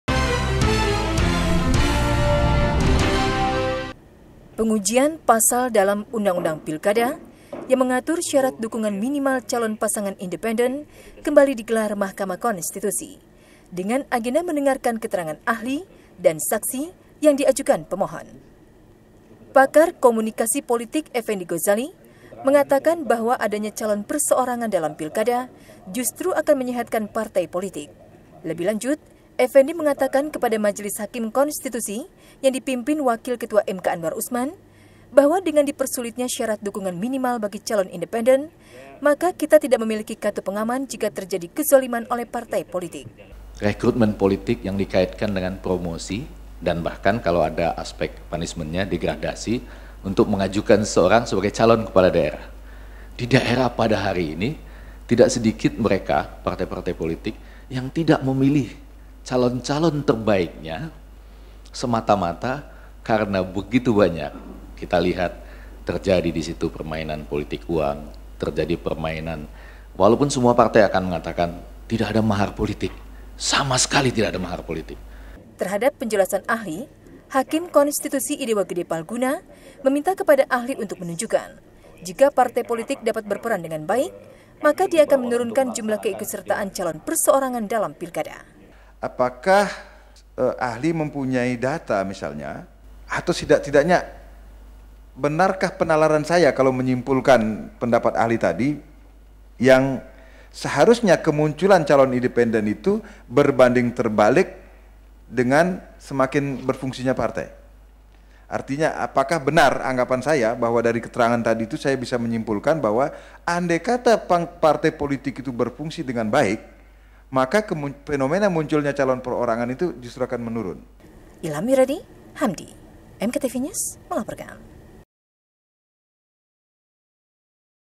10.07.15 Pengujian Undang-Undang Pilkada
Ahli : Keberadaan Calon Independen, Dapat Menyegatkan Partai Politik.